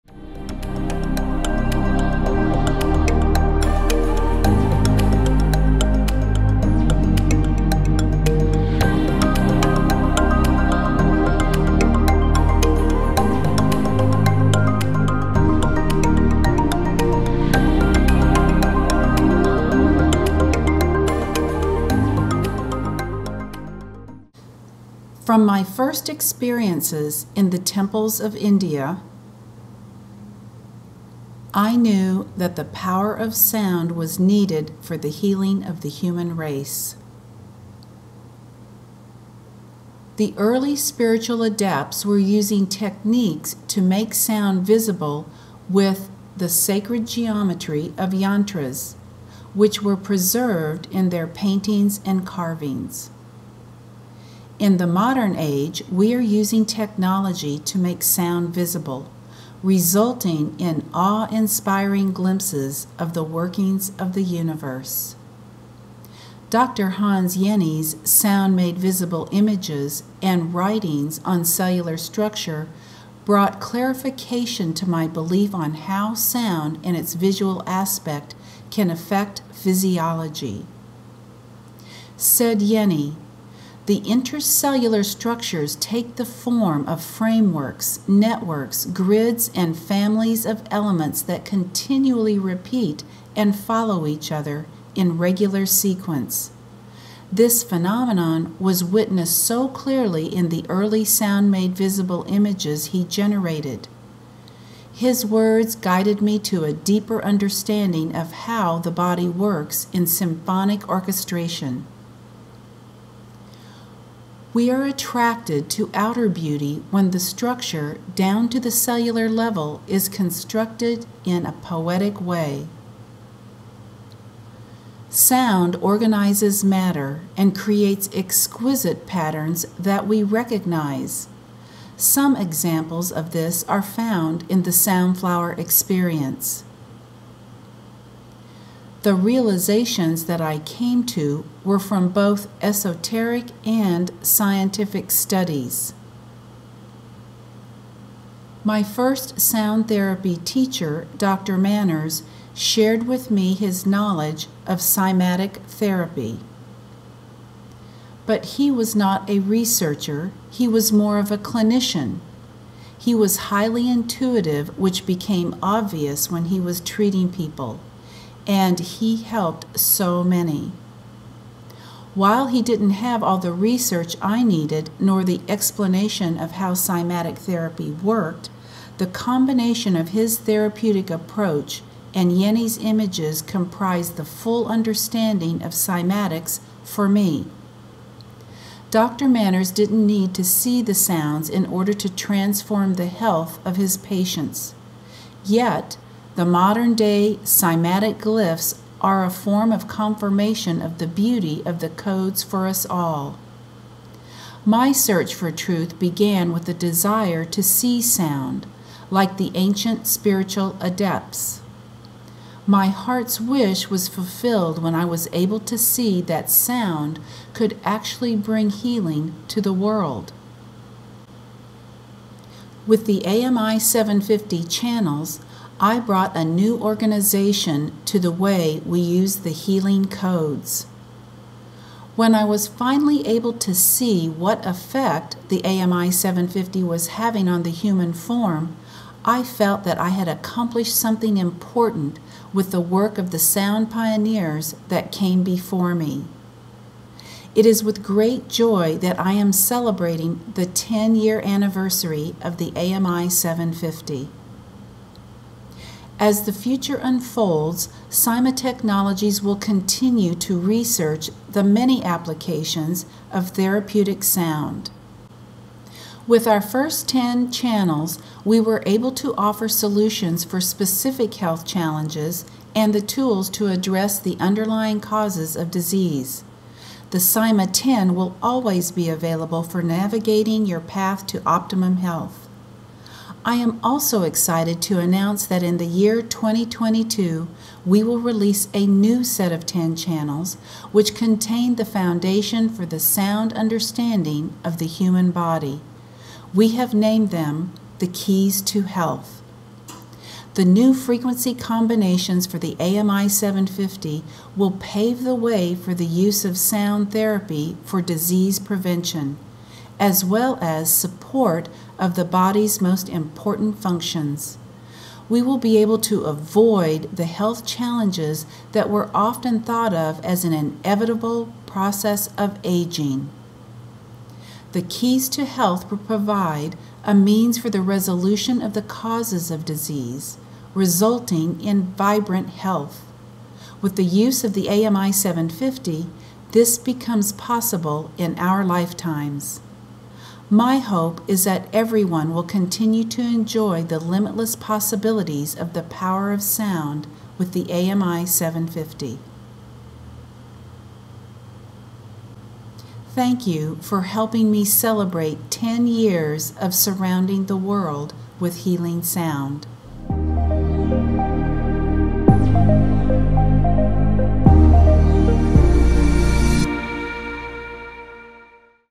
Part Ten—Looking Back Brings Clarity
Listen to the recollections of CEO Mandara Cromwell as she revisits her sound healing journey and shares revelations of the future of sound therapy.